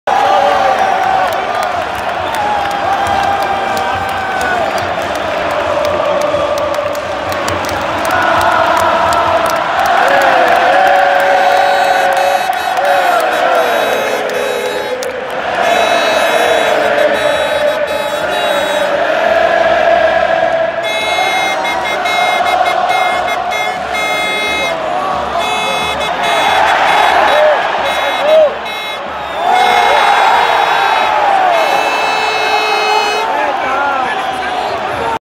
DIEGO SIMEONE MOTIVATION
Diego Simeone trying to get the crowd going against Barcelona.